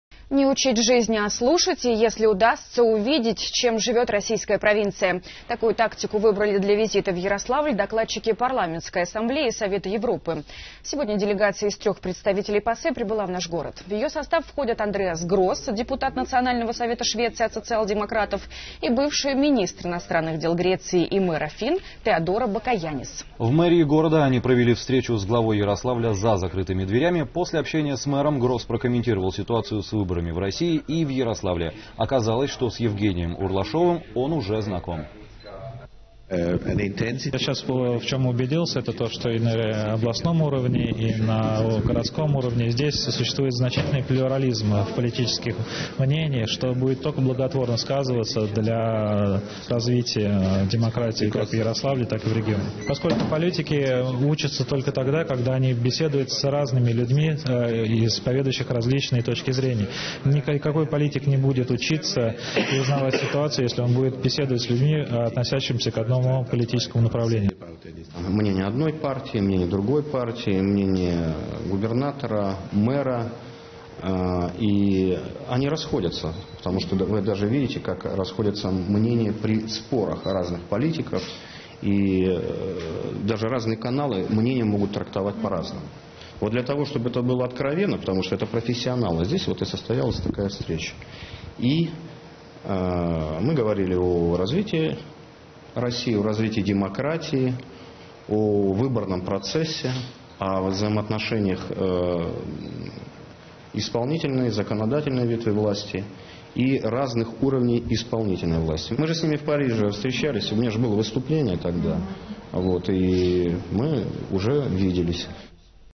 Делегация из трех представителей ПАСЕ провела встречу с мэром Ярославля «за закрытыми дверями»
Не учить жизни, а слушать. И, если удастся, увидеть, чем живёт российская провинция. Такую тактику выбрали для визита в Ярославль докладчики Парламентской ассамблеи Совета Европы. Делегация из трёх представителей ПАСЕ прибыла в наш город. В её состав входят Андреас Гросс - депутат Национального совета Швеции от социал-демократов и бывшая министр иностранных дел Греции и мэр Афин Теодора Бакояннис.